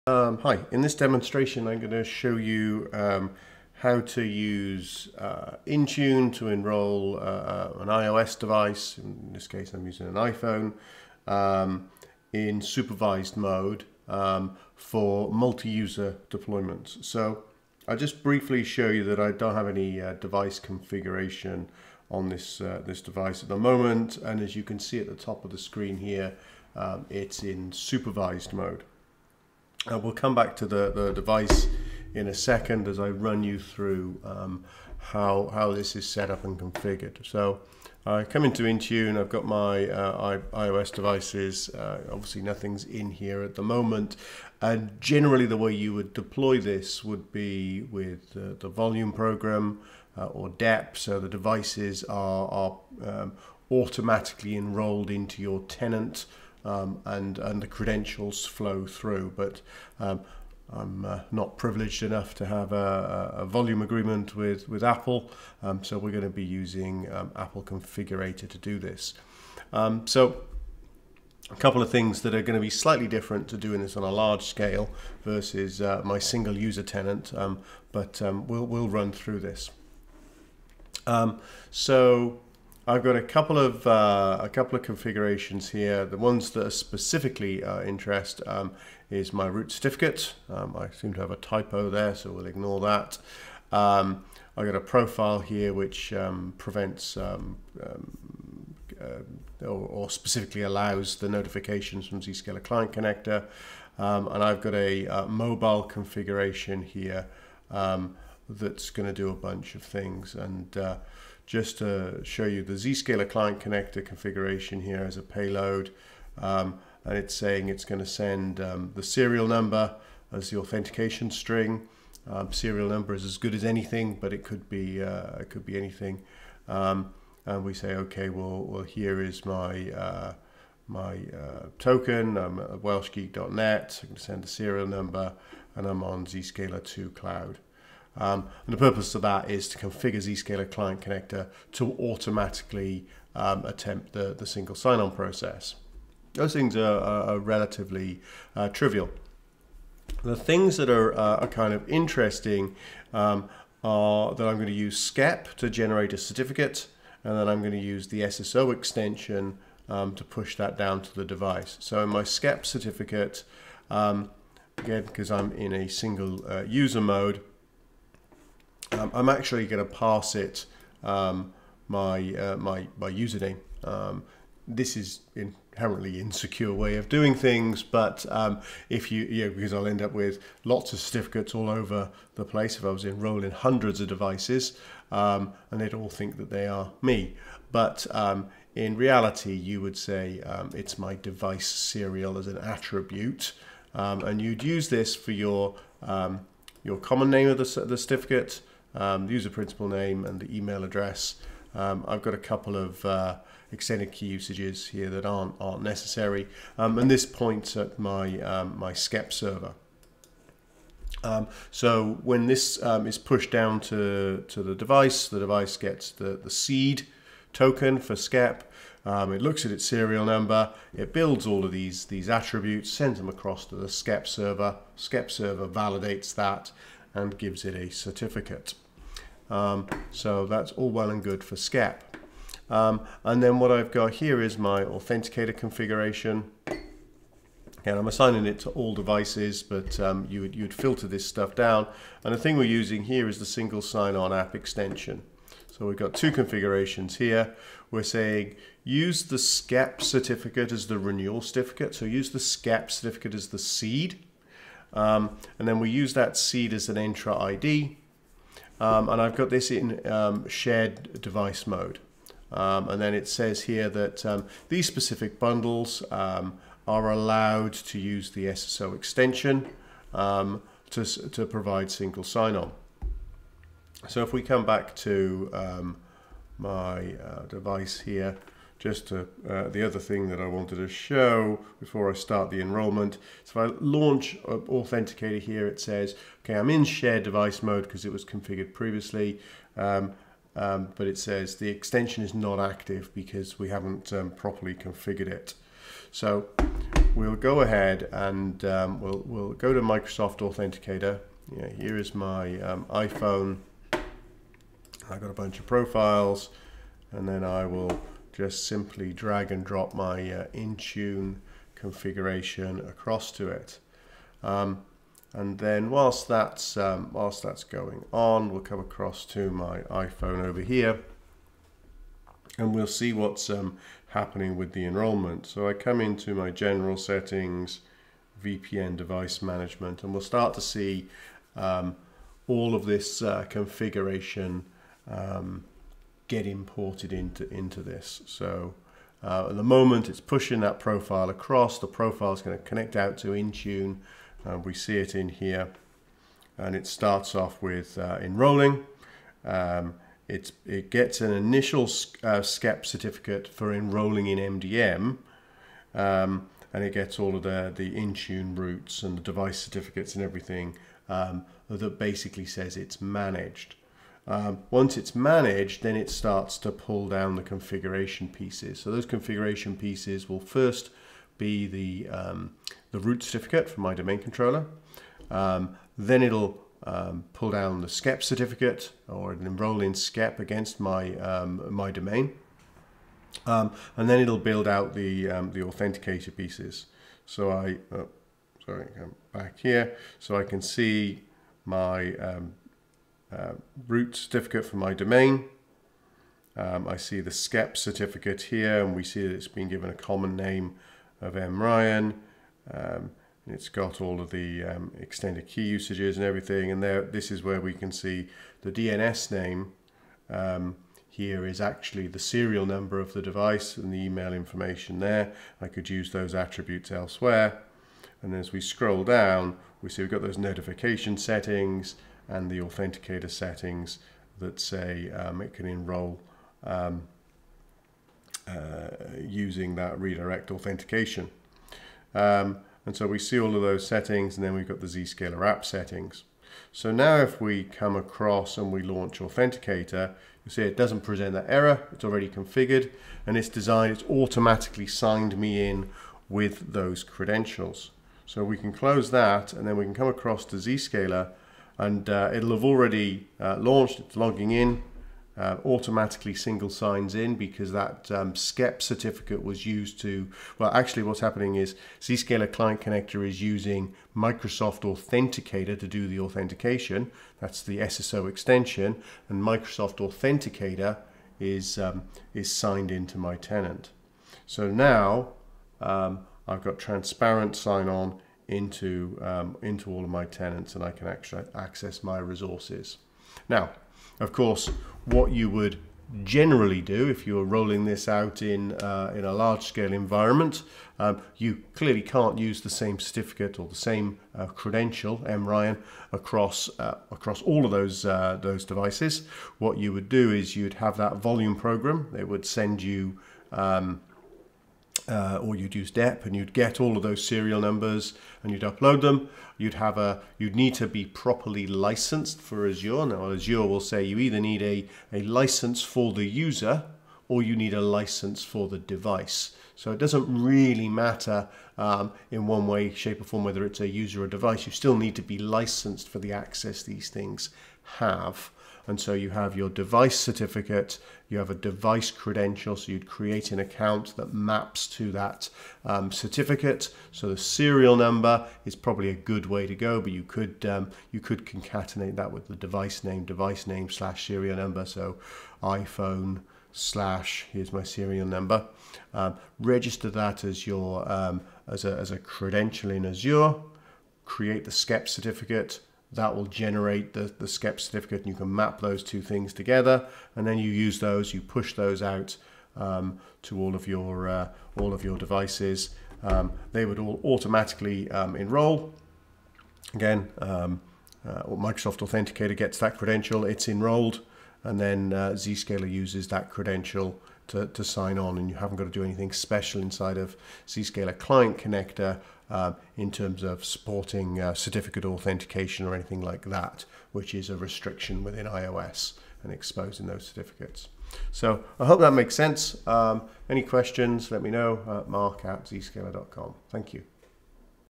Zscaler Client Connector - IOS, Supervised and Intune
In this session I demonstrate how to use SCEP certificates as a surrogate for Microsoft Authenticator App.  The SCEP certificates are hard-coded with a UPN, however in a production deployment you would use DEP or VPP to retrieve the Serial Number of the device and register that for the SCEP certificate.
The SCEP cert is used for SSO into the Microsoft Authenticator App for the SSO Extension.  This is set in Shared Device Mode, meaning that it's locked down, and cannot be changed by the end user.

Now that SCEP and Authenticator are configured, we configure Zscaler Client Connector to use Authenticator for SSO.  ZCC is deployed with the appropriate payload to register against the company tenant and present the SerialNumber as it's token, along with SSO against MS Authenticator.

In this way we can automatically deploy ZCC and Authenticator on Shared Devices, and avoid any user interaction, to get Zero Trust Access to applications.